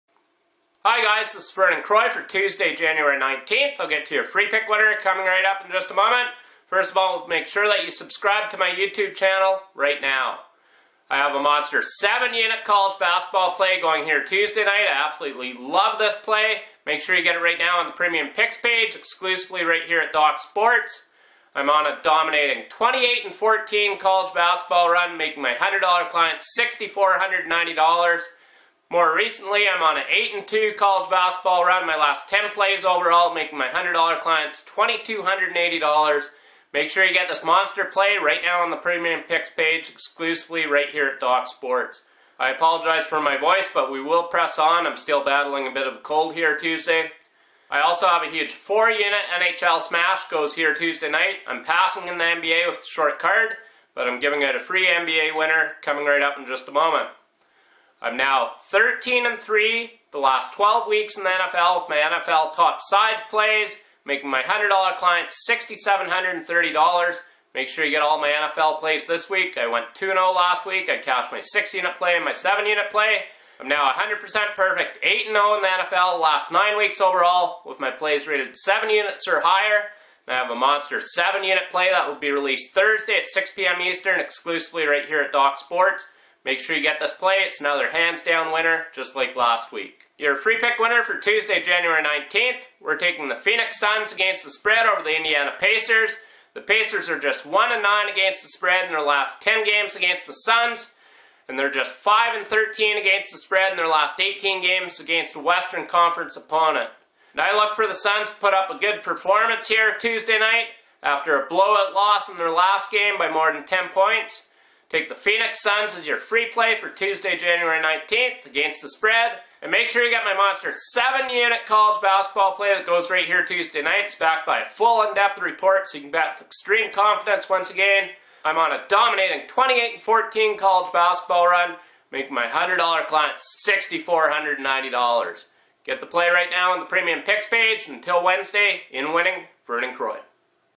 Free NBA pick Indiana Pacers vs Phoenix Suns 01/19/16 Vernon Croy
Todays free NBA pick is from handicapping expert Vernon Croy Your free NBA winner for Tuesday, January 19, 2016 is between the Indiana Pacers vs Phoenix Suns. to get more free NBA predictions each day as well as $60 free of Croy's premium member only predictions. Croy is on a huge 40-33 free pick video run!